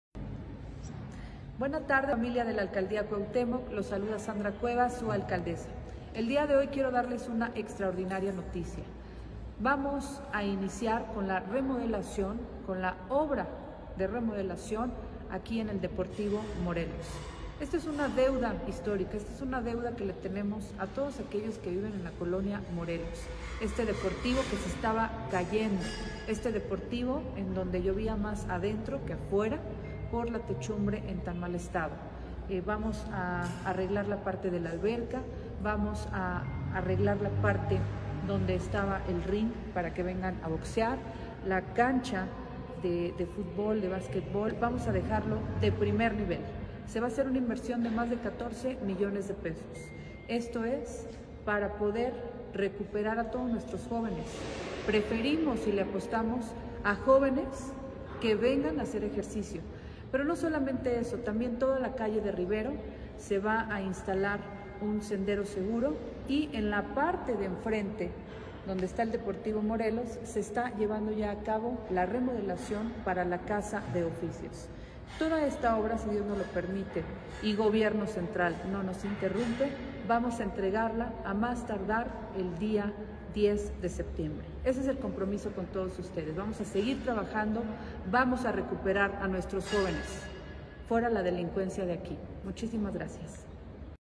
Hoy iniciamos la obra de remodelación del "Deportivo Morelos" en Cuauhtémoc 🏊🏋️⛹️🤾
Hoy iniciamos la obra de remodelación del "Deportivo Morelos" en Cuauhtémoc, con una inversión de más de 14 millones de pesos para que el BarrioDeTepito vuelva a ser semillero de deportistas de alto rendimiento y así disminuir la inseguridad y la pobreza.